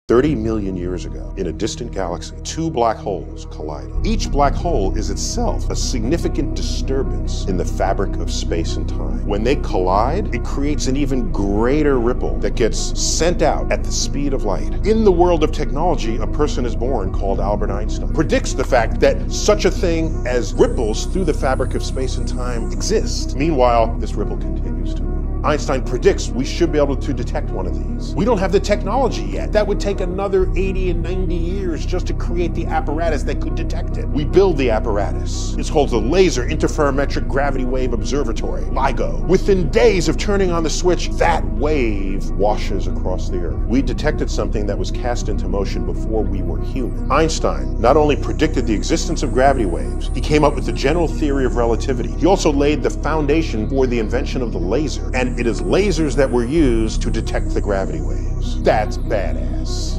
When Einstein Predicted Gravity Waves 🤯 w/ Neil deGrasse Tyson
Thirty million years ago, two black holes collided and sent out a massive ripple through space-time. 🌌 Einstein predicted these waves long before we had the tech to detect them. 📘 After nearly a century, LIGO was built — and it finally heard the universe. 🚀 This cosmic signal began long before humans even existed. ✨

This content is shared solely for educational and awareness purposes and is not the property of Cosmic Whispers of Time.